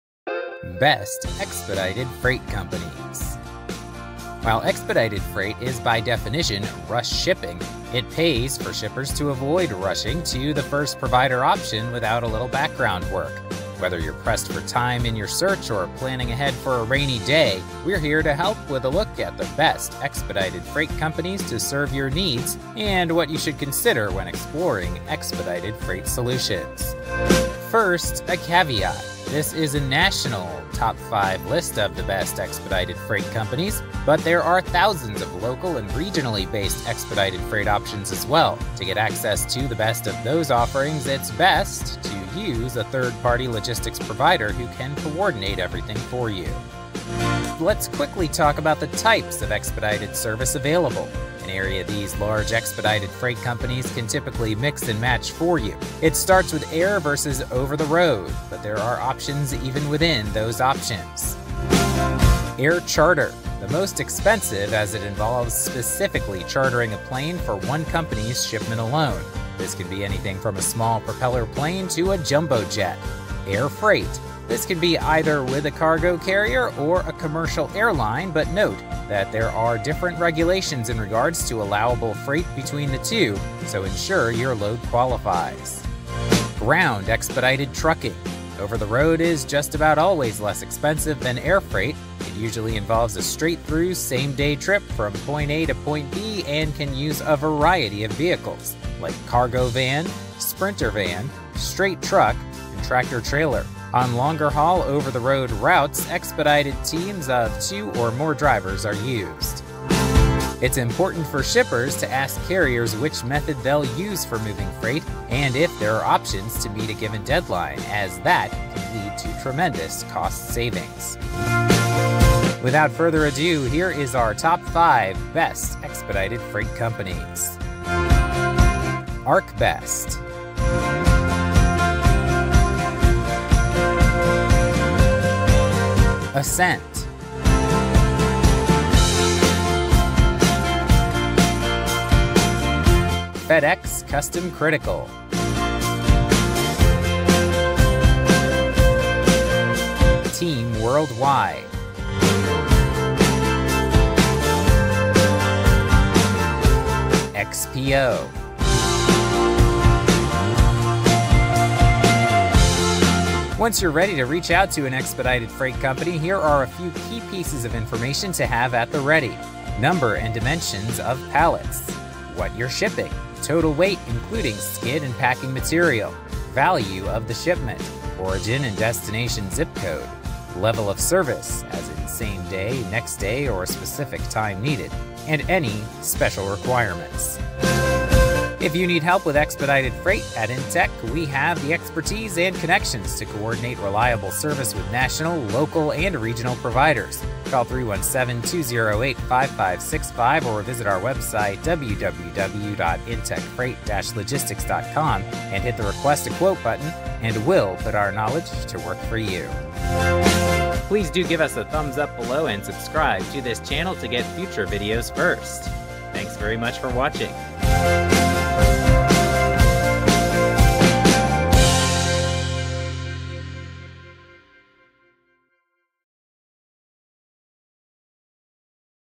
Best Expedited Freight Companies - A Top 5 List
Learn the best expedited freight companies to assist shippers in moving their cargo fast. In this video, you'll not only see a top 5 list of the best expedited freight companies, but you'll see the types of expedited freight, both air and over the road, as well as what information you should have ready when shopping for one.

For more in depth information on the best expedited freight companies,